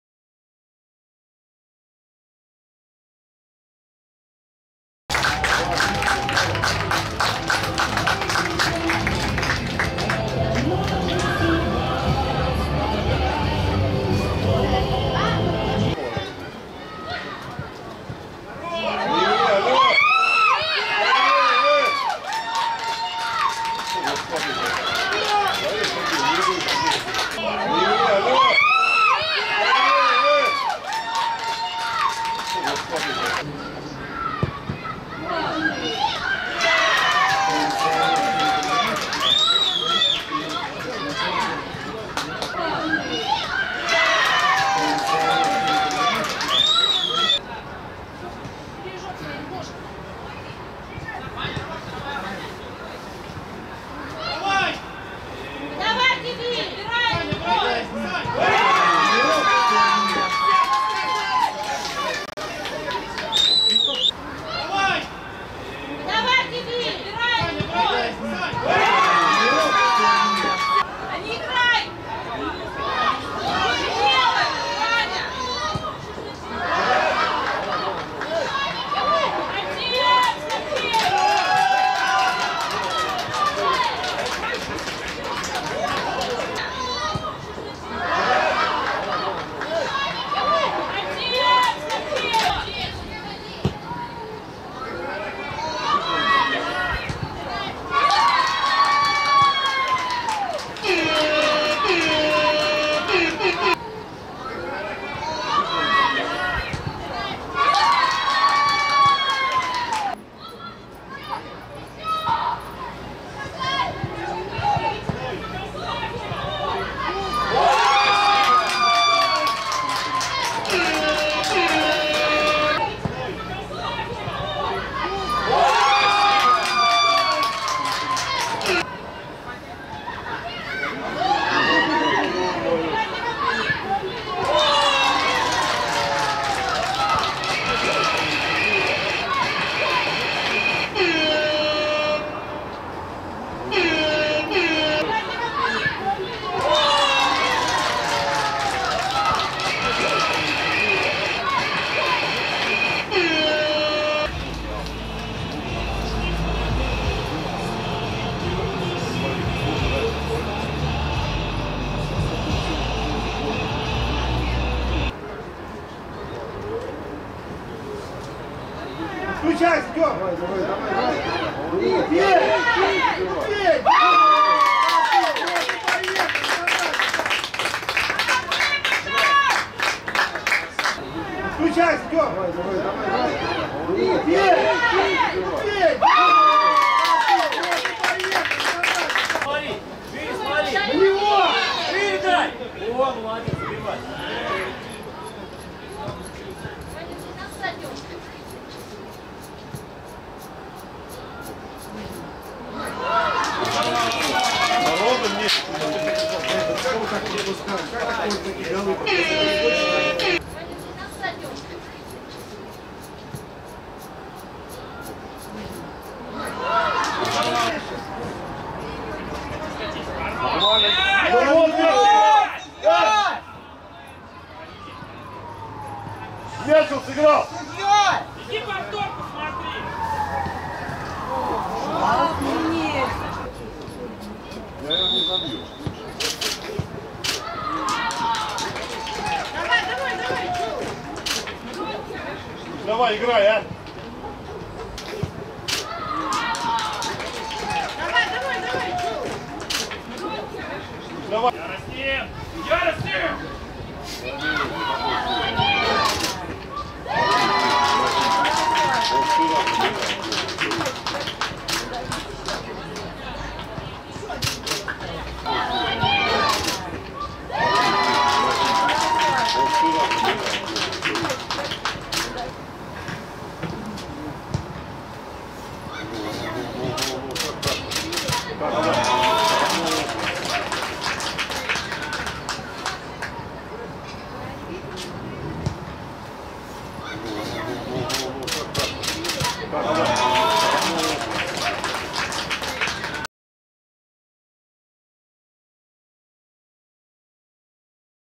«Спартак» — ФШМ (команды 2011 г. р.) — 7:0 (4:1 вторые составы)
Летнее первенство Москвы | 3-й тур
Команды 2011 года рождения
«Спартак» — ФШМ — 7:0 (4:0) (первые составы)
Голы: Галко, Родионов — 3, Хабаров, Балашев, Жеглов.
4:1 (вторые составы)
Голы: Абубакиров — 2, Романец (пен.), Жеглов.